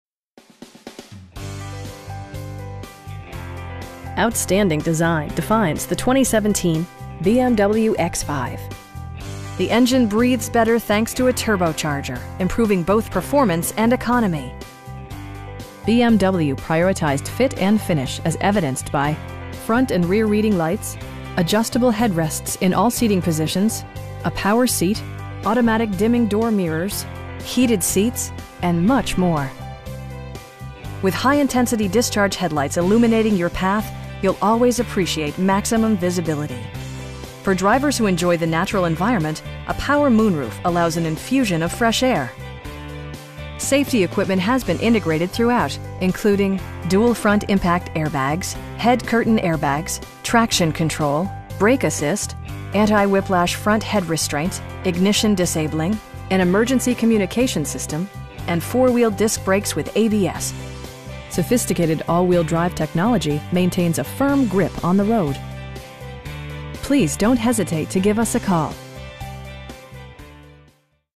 2017 BMW X5 xDrive35i in Highlands Ranch, CO 80129
Schomp BMW
1190 Plum Valley Lane

Outstanding design defines the 2017 BMW X5 The engine breathes better thanks to a turbocharger, improving both performance and economy. BMW prioritized fit and finish as evidenced by: front and rear reading lights, adjustable headrests in all seating positions, a power seat, automatic dimming door mirrors, heated seats, power windows, and much more. With high intensity discharge headlights illuminating your path, you'll always appreciate maximum visibility. For drivers who enjoy the natural environment, a power moon roof allows an infusion of fresh air. Safety equipment has been integrated throughout, including: dual front impact airbags, head curtain airbags, traction control, brake assist, anti-whiplash front head restraints, ignition disabling, an emergency communication system, and 4 wheel disc brakes with ABS. Sophisticated all wheel drive technology maintains a firm grip on the road. We pride ourselves on providing excellent customer service. Please don't hesitate to give us a call.